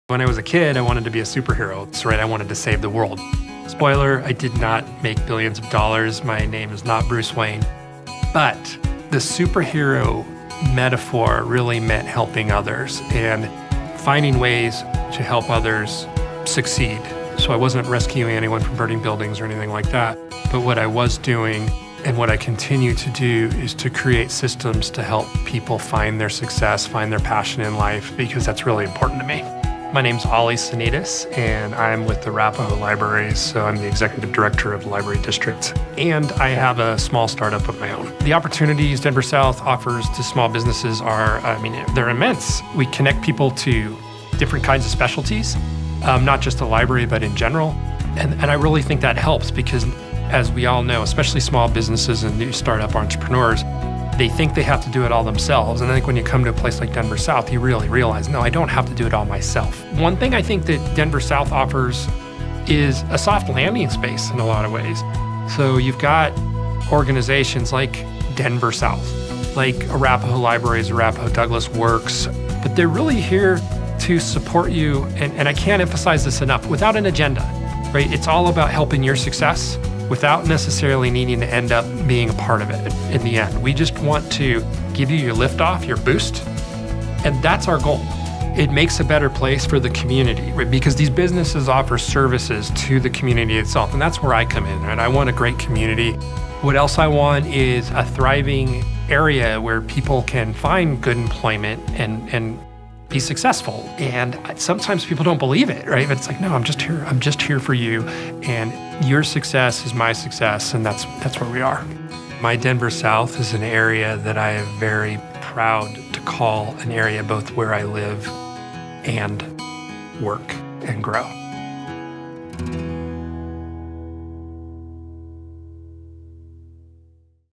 My Denver South - Oli Sanidas, Arapahoe Libraries
When he was a kid Oli Sanidas, Executive Director of Arapahoe Libraries, wanted to be a superhero. Although this career ambition never came to fruition, Oli has been able to leverage his passion for helping other people through his career at Arapahoe Libraries, where he has the opportunity to create systems to help others find their success and passion in life.